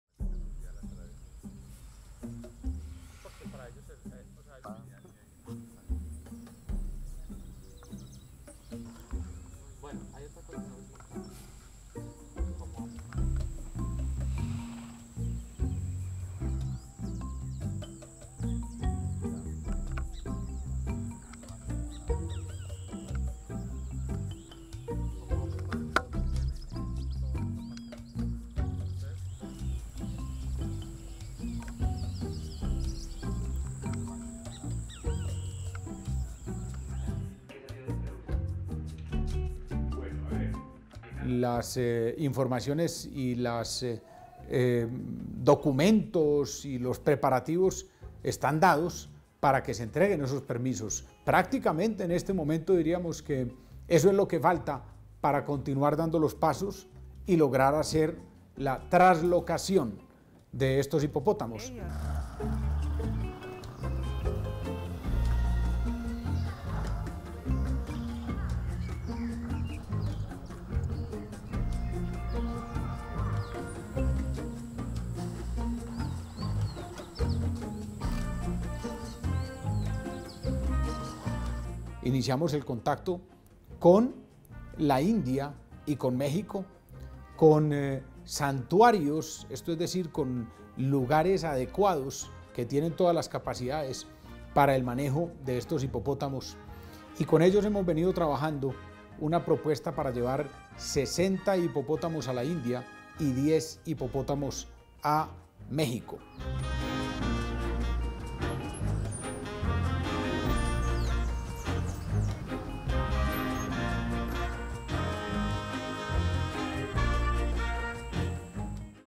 Colombia quiere deshacerse de la mitad de los hipopótamos de Pablo Escobar
En el departamento de Antioquia, en Colombia, se crían sin control unos 150 hipopótamos. Son los descendientes de los animales del zoológico personal del extinto capo del narcotráfico Pablo Escobar. Las autoridades regionales se muestran optimistas ante la posibilidad de que el Gobierno colombiano dé luz verde al envío de la mitad de estos animales a santuarios de India y México en los próximos meses luego de que la presencia de los imponentes cuadrúpedos se convirtiera en un dolor de cabeza para los pobladores.